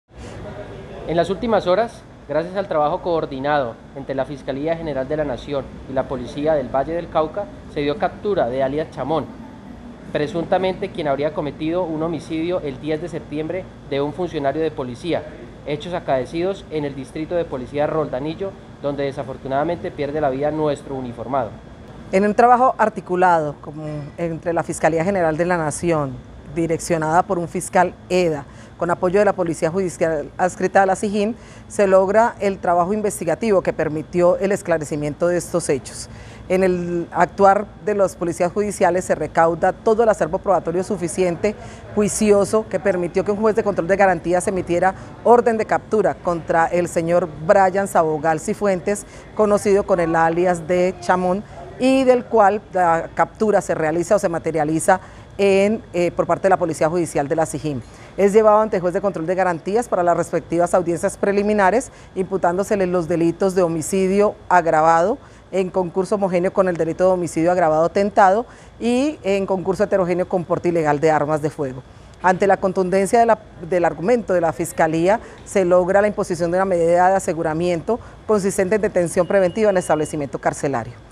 Capturado alias ‘CHAMÓN’ vinculado en el homicidio de un intendente de policía
La policía nacional capturó en Roldanillo a un hombre de 21 años de edad vinculado en el homicidio de un intendente de la policía por hechos ocurridos el día 10 de septiembre del presente año.